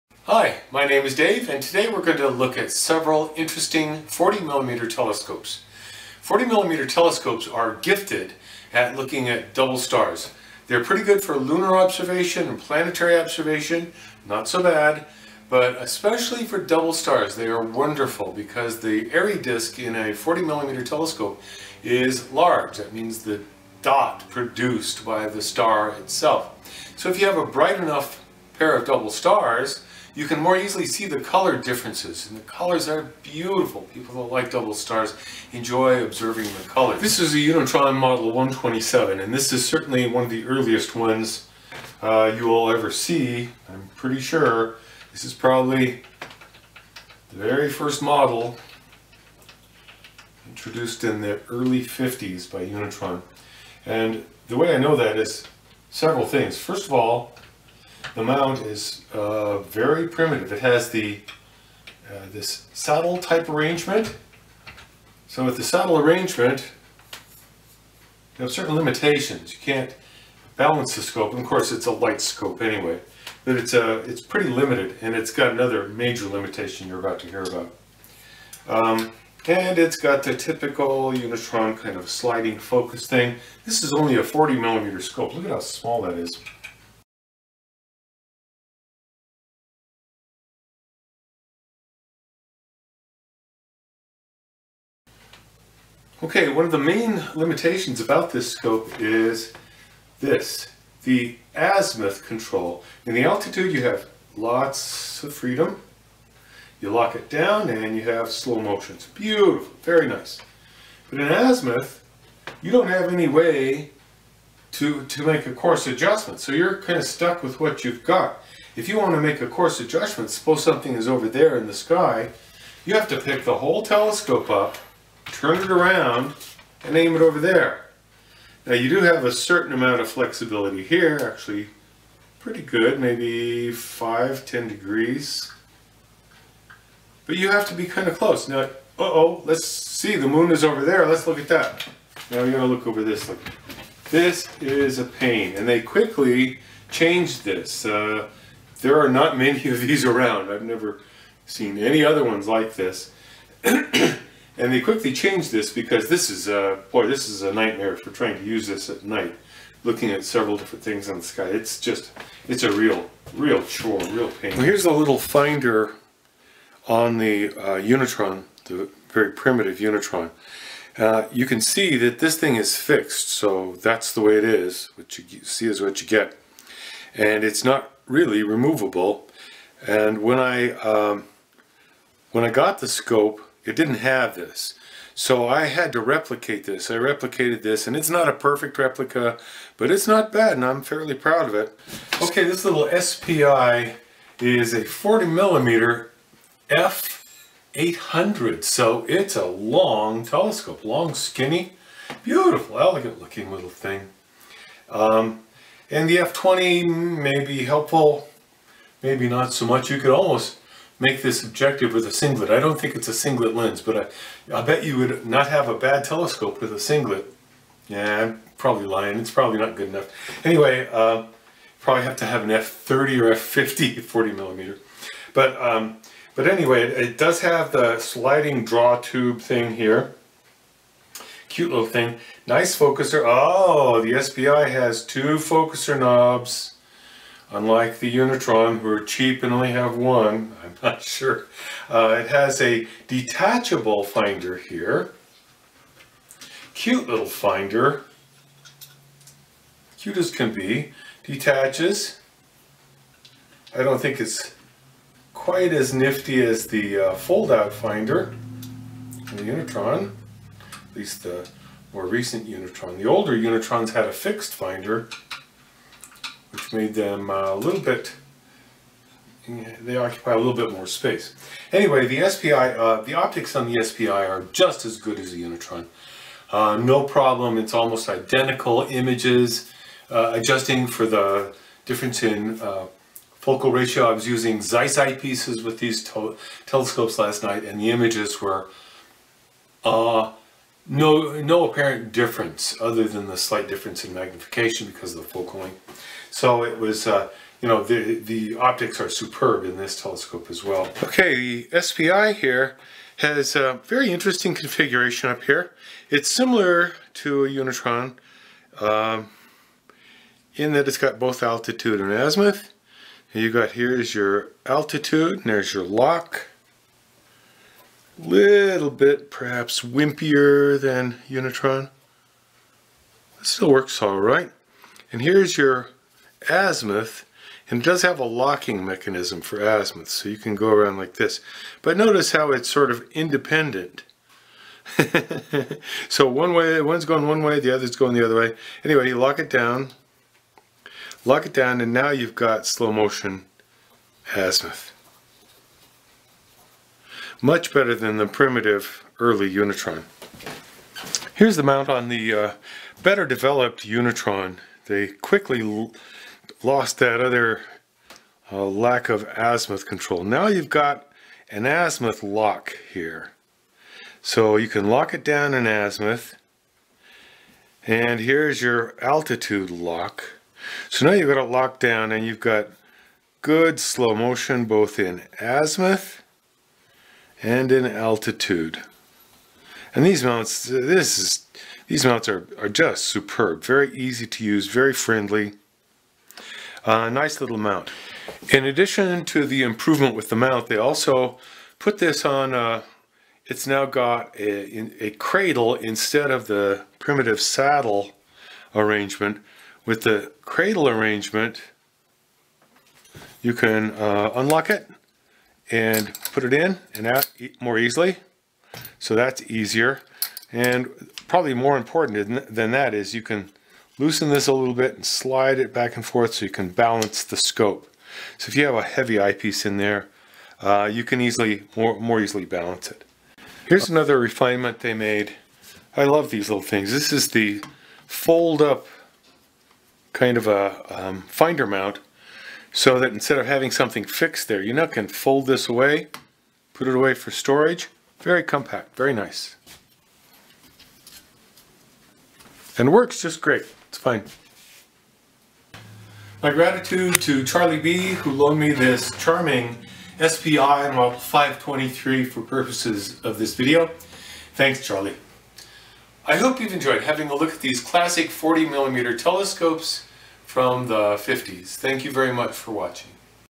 Classic 40mm Telescopes
Direct from the 1950's here is a look at some of the smallest aperture telescopes that are useful for astronomy. These are more rare than the comparatively gigantic 60mm scopes. They have a certain captivating charm.